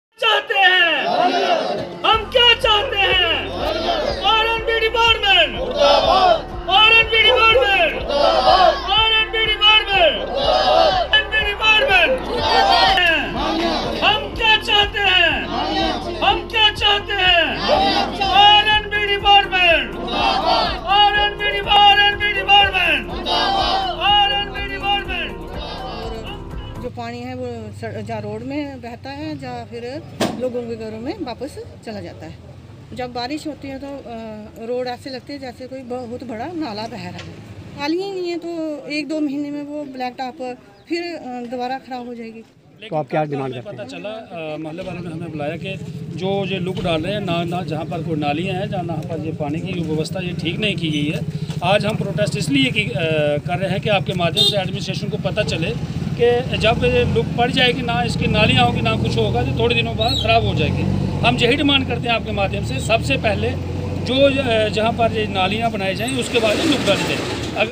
सड़क पर नालियों का काम अधर में छोड़कर सड़क पर तारकोल बिछाने का कार्य करवाया जा रहा है जिसके विरोध म
ऊधमपुर : सड़क पर नालियों का काम अधर में छोड़कर सड़क पर तारकोल बिछाने का कार्य करवाया जा रहा है जिसके विरोध में समाज सेवक गोविंद चलोत्रा की अध्यक्षता में भारत नगर वासियों में किया जोरदार किया जोरदार प्रदर्शन ।